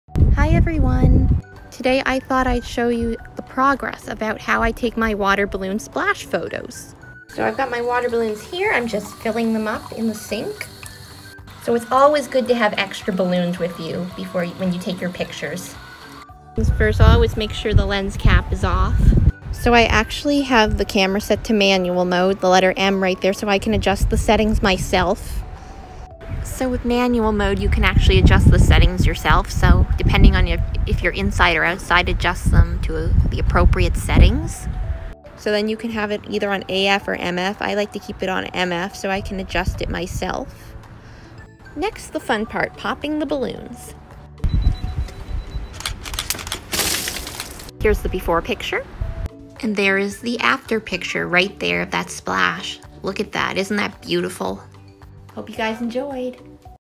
Water Balloon Splash Photo Tutorial 📸 🎈 🎯 💦
A little step by step progress on how I take some water balloon splash photos.